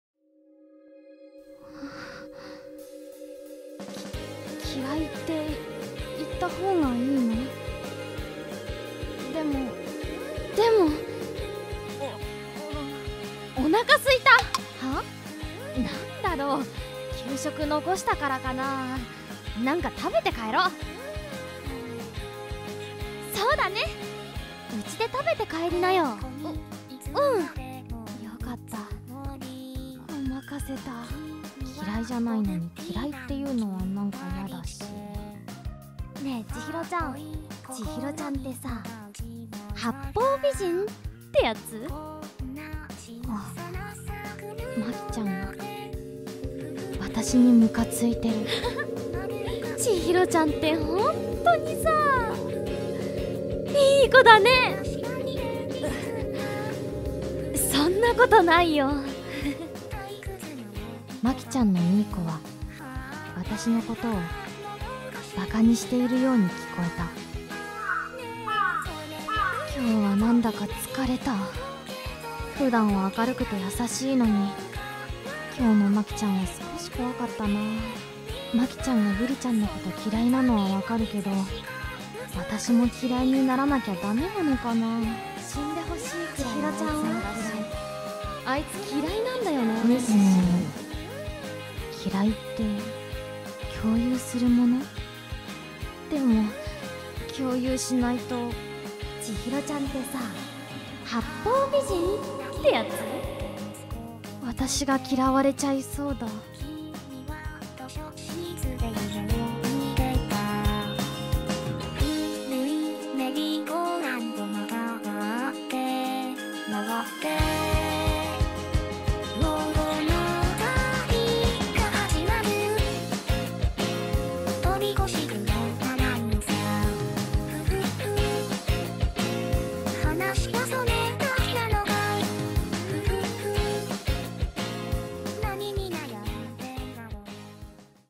『不登校ガール』第15話 「千尋は八方美人？ 女子同士の同調圧力に悩む千尋…」【ボイス漫画】
まんがアプリ「Vコミ」で大人気連載中の『不登校ガール』（園山千尋）15話を、声優さんのボイス付きで動画にしました。

第15話 「千尋は八方美人？ 女子同士の同調圧力に悩む千尋…」

・オリジナルの漫画は、Vコミで無料で読めるよ！

園山千尋さんは、朝日新聞のイベント「withyou」に参加しています。

Copyright:園山千尋 & Vコミ
voice:このみまこ